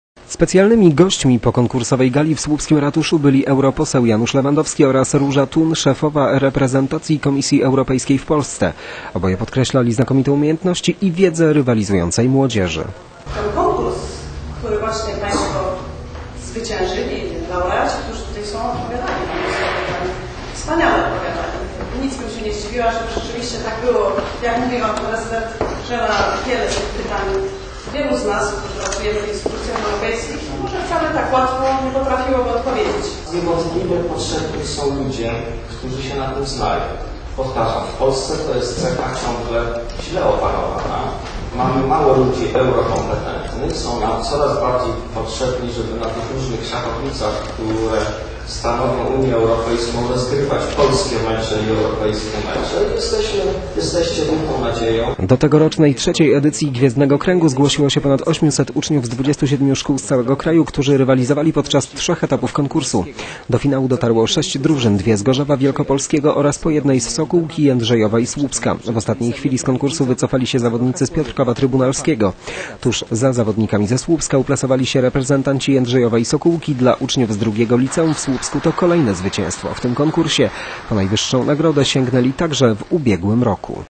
Słupscy uczniowie eurokompetentni
Reprezentanci II Liceum Ogólnokształcącego im. Adama Mickiewicza w Słupsku zajęli pierwsze miejsce w konkursie wiedzy o Unii Europejskiej „Gwiezdny krąg".  Zwycięzcy w nagrodę pojadą do Brukseli.